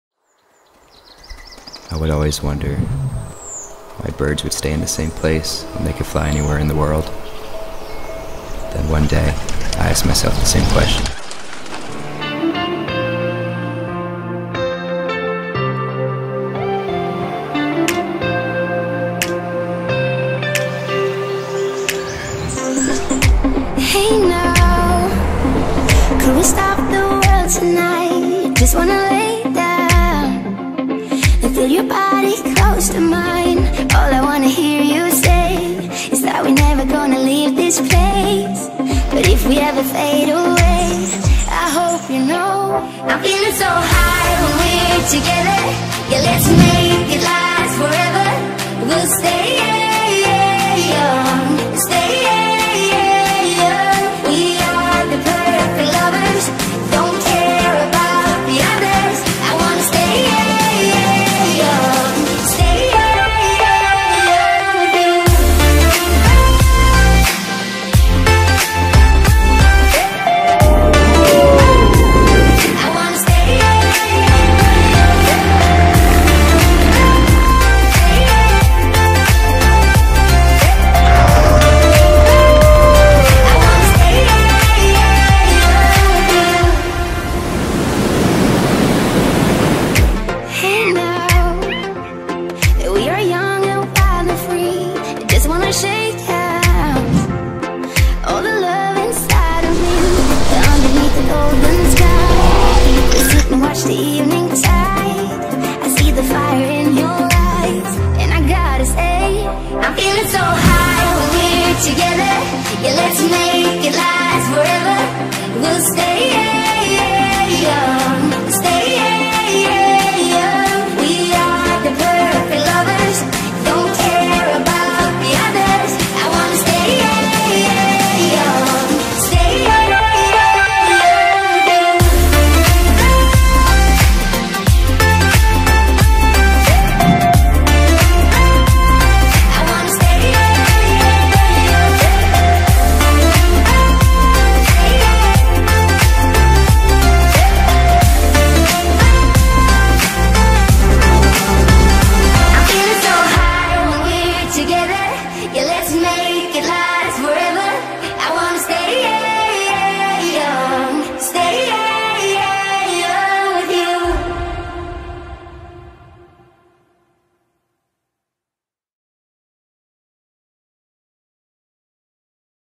Fly Away - In Search of Iguassu | Argentina to Brazil 2017
"I always wonder why birds would stay in the same place, when they can fly anywhere in the world. Then one day I asked myself the same question"

Inspired by this quote, I decided to escape the german winter and travel to South America in December 2017. Together with 13 international friends I took the journey from Buenos Aires (Argentina) to Rio (Brazil) crossing 3 countries, riding over 2500 km and collecting endless memories.

Thanks to everyone, who made this trip special - especially @Alex, Anna, Fleur, Caitlyn, Jason, Claudia, Marie, Robina, Shireen, Anne, Aura, Milja & Luz

Voice Layover by: @TaylorCutFilms
Videos by myself shot on: DJI Mavic Pro, Sony Alpha 6300, GoPro Hero 6